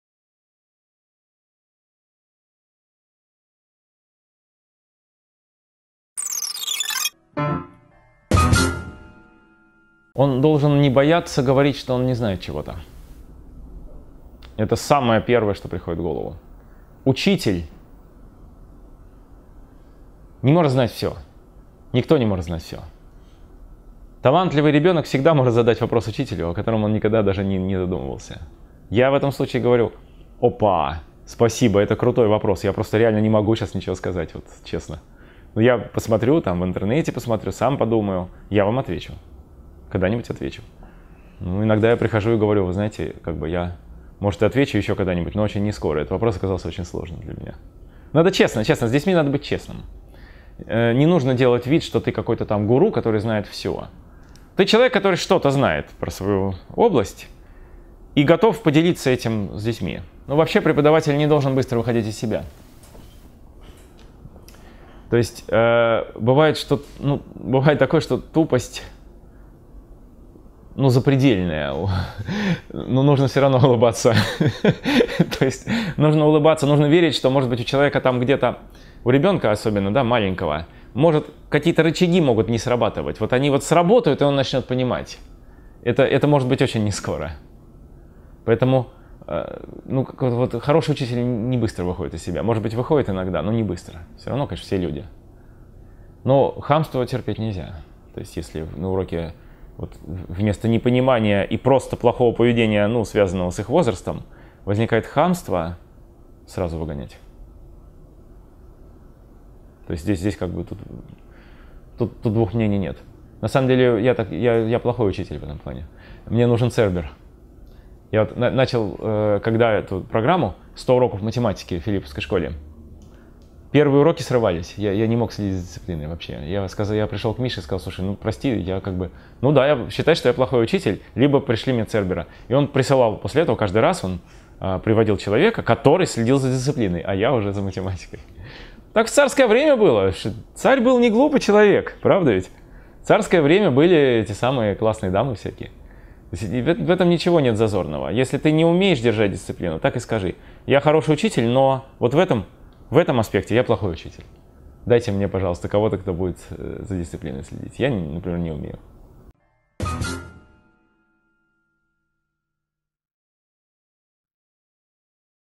Каким должен быть учитель
Герой МЫSLей - популяризатор науки - Алексей Савватеев. Доктор физико-математических наук, специалист в области теории игр. Работает одновременно в нескольких научных учреждениях, в том числе в Лаборатории исследования социальных отношений и многообразия общества РЭШ. Читает в Яндексе лекции в Школе Анализа Данных, участвует в теоретических исследованиях. В Иркутске на 0.2 ставки работает доцентом ИГУ. Алексей Савватеев организует ежегодную Школу междисциплинарного анализа социально-экономических процессов, Байкальские Чтения, Стекловский Десант в Иркутске, социально-политические семинары при будущем Университете Дмитрия Пожарского, начал читать “100 лекций по математике” в Филипповской Школе. Ведет “Математику для гуманитариев”.